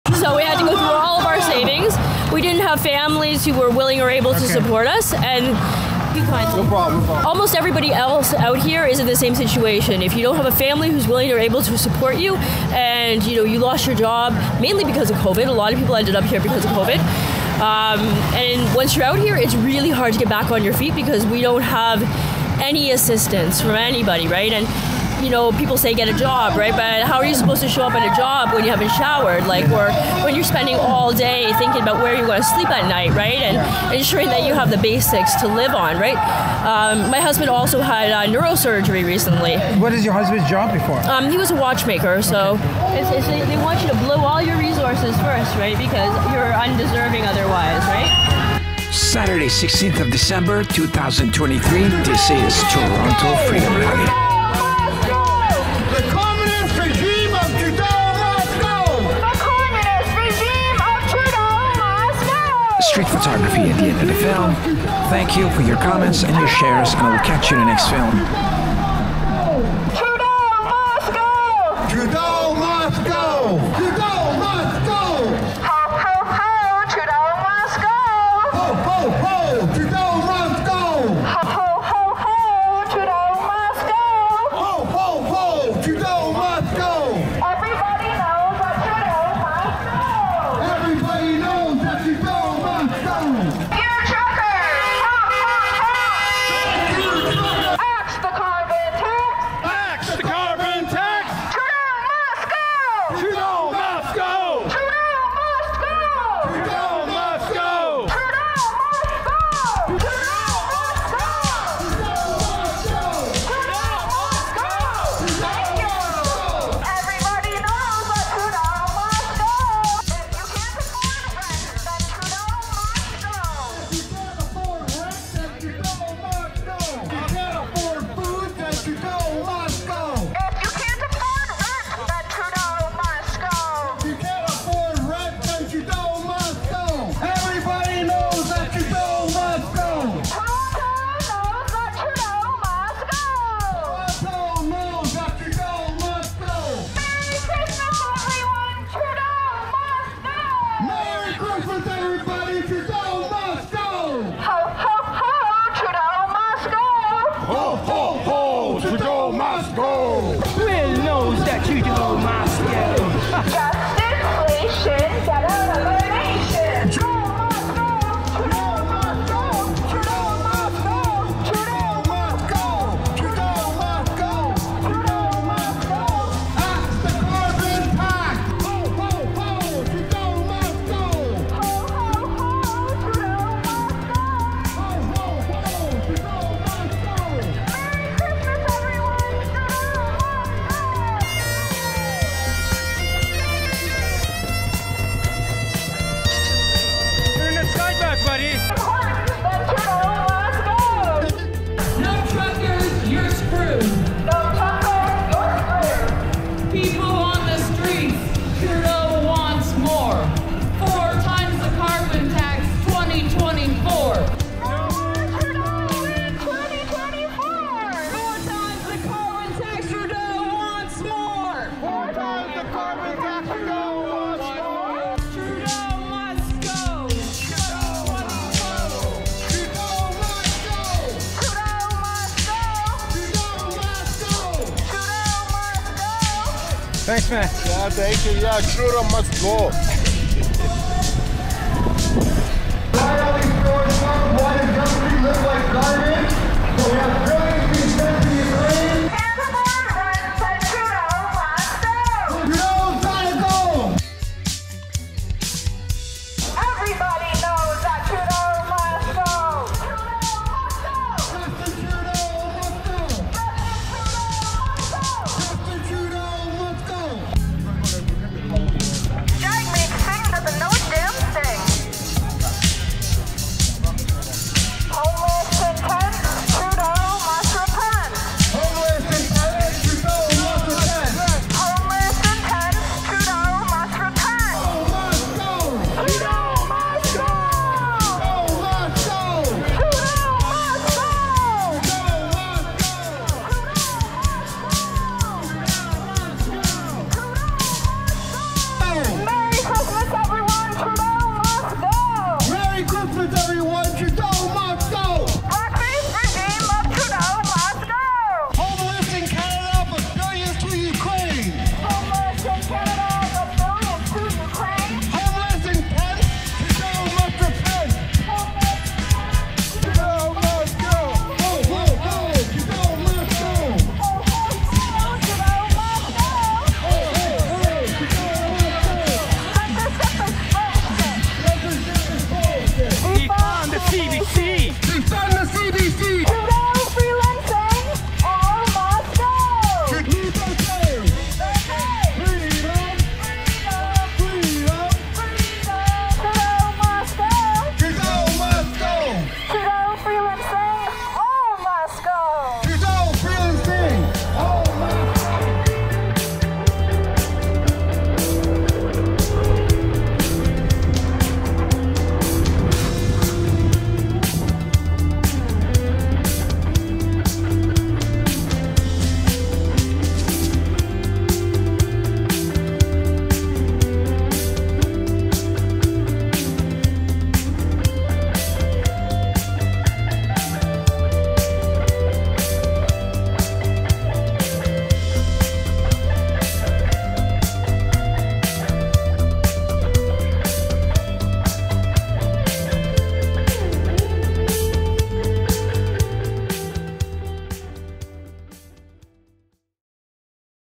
Homeless interview at Toronto Freedom Rally -7.2 Km
Dec 16 2023 - Interview with a newly homeless person on Yonge Street. StreetPhotography / Portraits art the end of the film - Cold & wet day - A short 7,2 km walk with torontoFreedomRally. Merry Christmas every one. Checking out Queens Park Building Christmas decorations at the end of the film - Please help people in need & Keep comments here YT friendly.